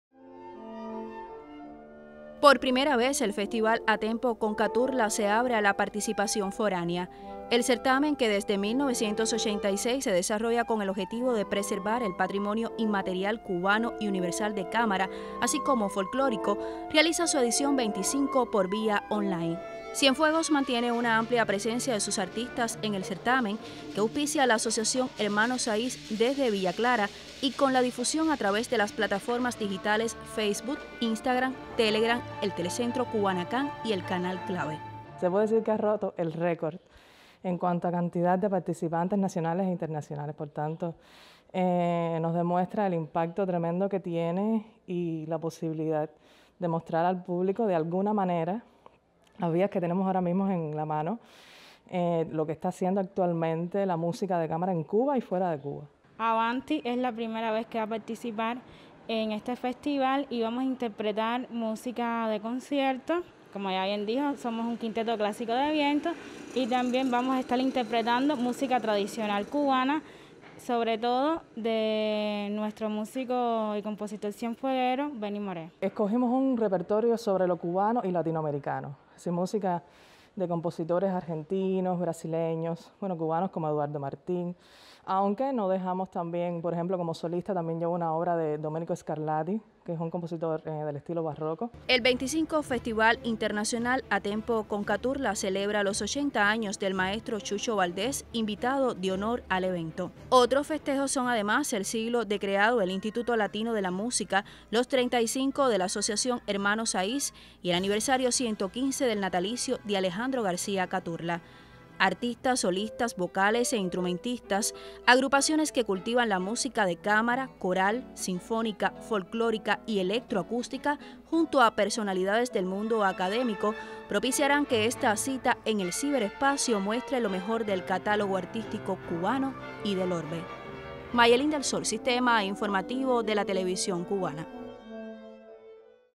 Participan músicos cienfuegueros en el 25 Festival ¨A Tempo con Caturla¨ que se efectúa vía online
El 25 Festival A tempo con Caturla de Música de Cámara, se celebra del 2 al 7 de marzo en Villa Clara, conjuntamente con la Asociación Hermanos Saíz. El evento que se efectuará por vía online permitirá promocionar las noveles agrupaciones que llegarán por primera vez al certamen, así como la labor de los más experimentados.